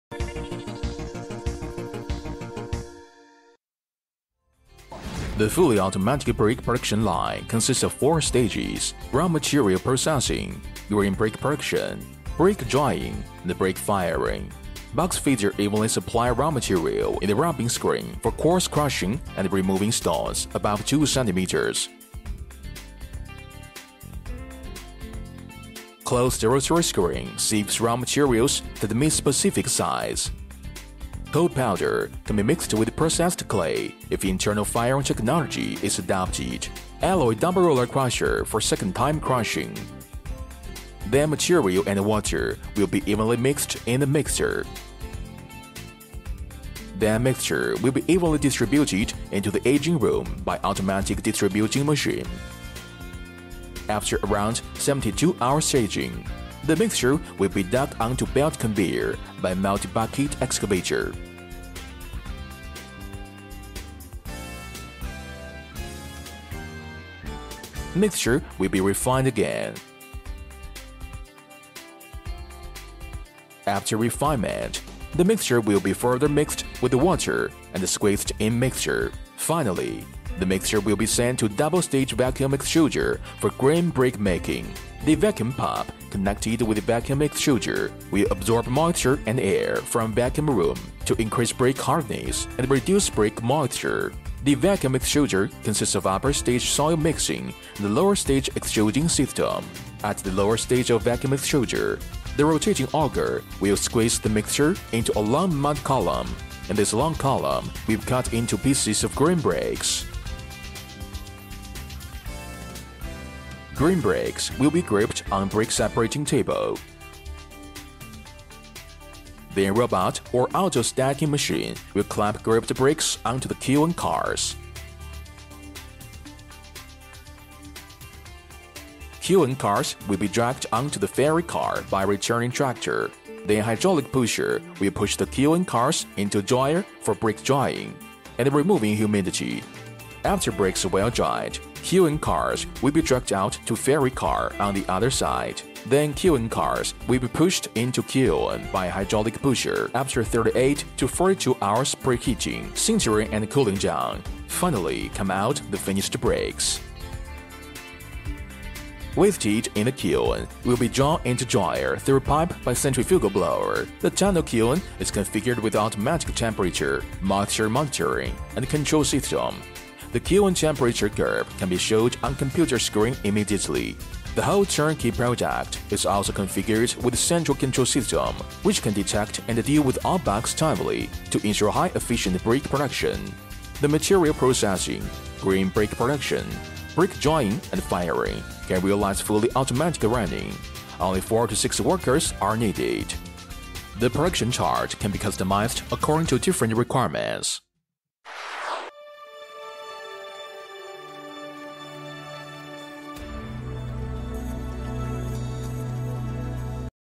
Yingfeng fully automatic tunnel kiln robot stacking factory clay brick making machine plant
The tunnel kiln is configured with automatic temperature ,moisture monitoring and control system .The kiln temperature curve can be showed on computer screen immediately.

From the kiln ,waste heat will be drawn into dryer through pipe and Centrifugal blower

The whole turn-key project is also configured with central control system which can defect and deal with all bugs timely to ensure high efficient brick production.
The material processing, green brick production ,brick drying and firing can realize fully automatic running, only need 4~6 operators.

The production chart can be customized according to different requirements.

Feng kai  ( from Yingfeng team)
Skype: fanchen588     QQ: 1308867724

Our mission is to support you to build a modern brick & tile plant.